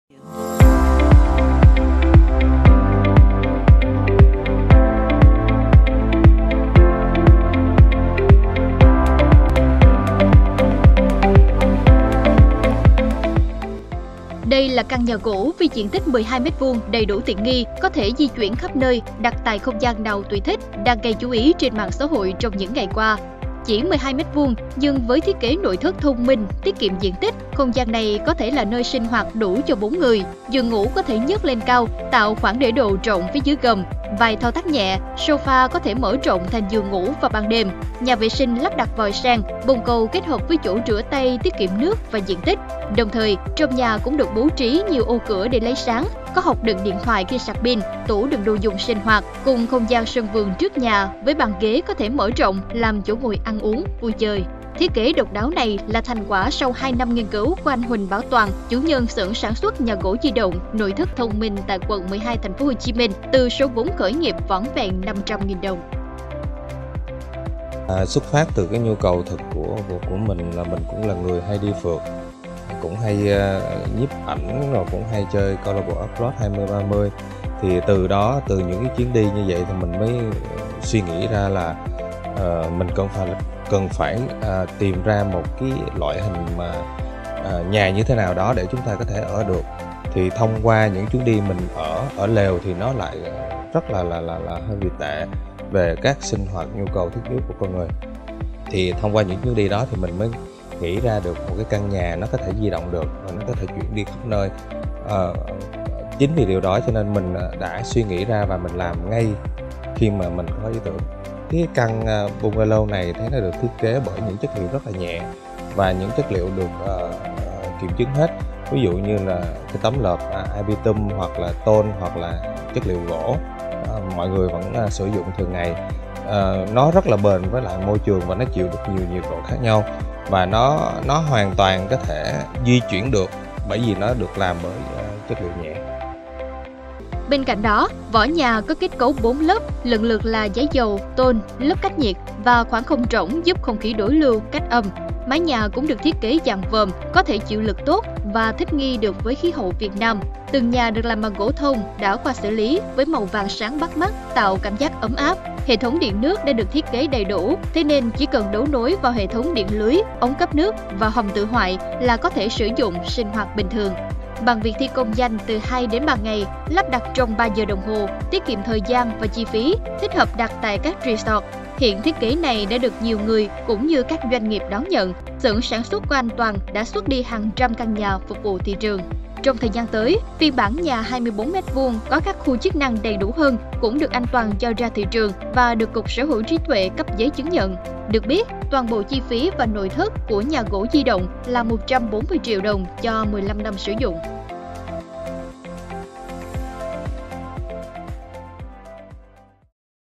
Độc đáo nhà gỗ di động giá 140 triệu đồng nhiều người mơ ước | NLĐTV
Đây là căn nhà gỗ di động với diện tích 12m2 đầy đủ tiện nghi có thể di chuyển khắp nơi, đặt tại không gian nào tùy thích, có giá chỉ 140 triệu đồng, đang gây chú ý trên mạng xã hội trong những ngày qua.

Chỉ 12m2 nhưng với thiết kế nội thất thông minh, tiết kiệm diện tích, không gian của nhà gỗ di động này có thể là nơi sinh hoạt đủ cho 4 người. Giường ngủ có thể nhấc lên cao tạo khoảng để đồ rộng phía dưới gầm, vài thao tác kéo nhẹ sofa cũng có thể mở rộng thành giường ngủ vào ban đêm, nhà vệ sinh lắp đặt vòi sen, bồn cầu kết hợp với chỗ rửa tay tiết kiệm nước và diện tích. Đồng thời, trong nhà cũng bố trí nhiều ô cửa lấy sáng, có hộc đựng điện thoại khi sạc pin, tủ đựng đồ dùng sinh hoạt… cùng không gian sân vườn trước nhà với bàn ghế có thể mở rộng làm chỗ ngồi ăn uống, vui chơi.

Thiết kế nhà gỗ di động độc đáo này là thành quả sau 2 năm nghiên cứu của anh Huỳnh Bảo Toàn – chủ nhân xưởng sản xuất nhà gỗ di động, nội thất thông minh tại quận 12, TP HCM.

Bên cạnh đó, vỏ nhà gỗ di động có kết cấu bốn lớp lần lượt là giấy dầu, tôn, lớp cách nhiệt và khoảng không rỗng giúp không khí đối lưu, cách âm. Mái nhà cũng thiết kế dạng vòm nên có thể chịu lực tốt và thích nghi được khí hậu Việt Nam. Tường nhà được làm bằng gỗ thông đã qua xử lý với màu vàng sáng bắt mắt, tạo cảm giác ấm áp.

Hệ thống điện nước đã được thiết kế đầy đủ thế nên chỉ cần đấu nối vào hệ thống điện lưới, ống cấp nước và hầm tự hoại là có thể sử dụng, sinh hoạt bình thường.

Bằng việc thi công nhanh từ 2-3 ngày, lắp trong 3 giờ đồng hồ, tiết kiệm thời gian và chi phí, thích hợp đặt tại các resort, hiện thiết kế này đã được nhiều người cũng như các doanh nghiệp đón nhận. Xưởng sản xuất của a Toàn đã xuất đi hàng trăm căn nhà phục vụ thị trường.

Trong thời gian tới, phiên bản nhà gỗ di động 24 m2 có các khu chức năng đầy đủ hơn cũng được anh Toàn cho ra thị trường và được cục sở hữu trí tuệ cấp giấy chứng nhận.

Được biết, toàn bộ chi phí và nội thất của nhà gỗ di động là 140 triệu đồng cho 15 năm sử dụng.